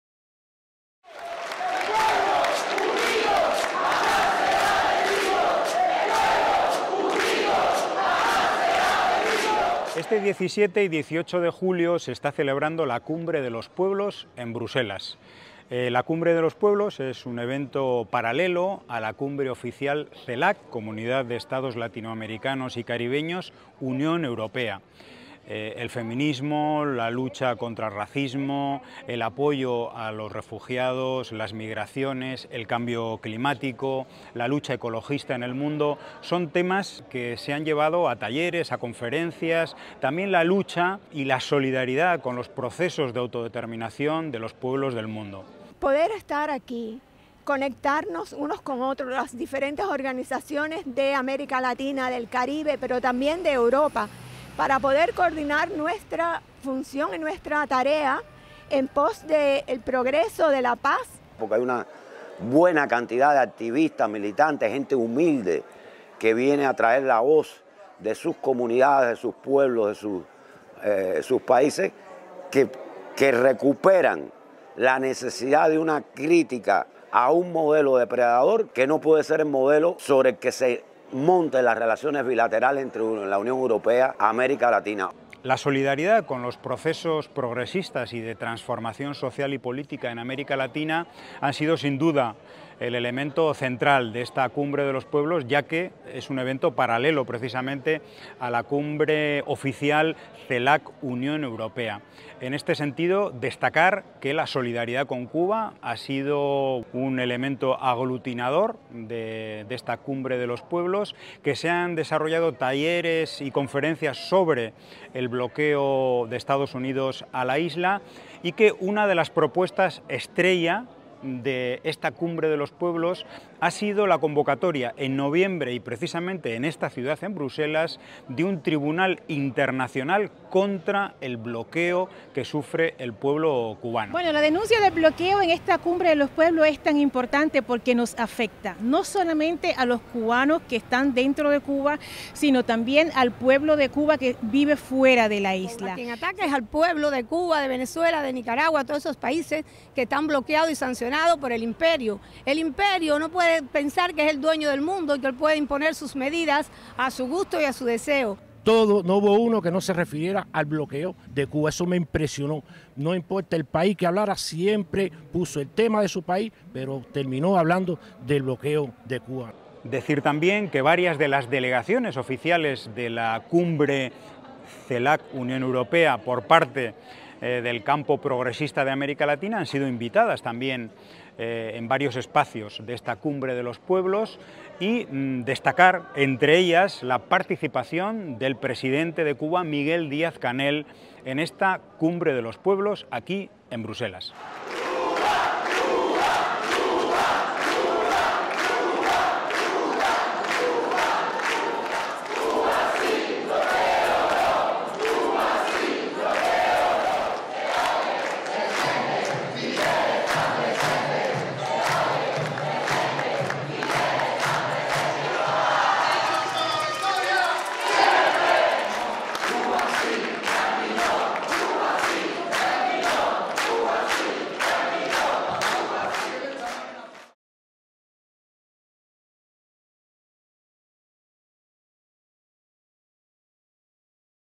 La denuncia del bloqueo a Cuba fue elemento aglutinador de la Cumbre de los Pueblos en Bruselas
Cubainformación.- La denuncia del bloqueo a Cuba fue uno de los elementos aglutinadores de la Cumbre de los Pueblos, celebrada en Bruselas los días 17 y 18 de julio de forma paralela a la Cumbre oficial CELAC-Unión Europea.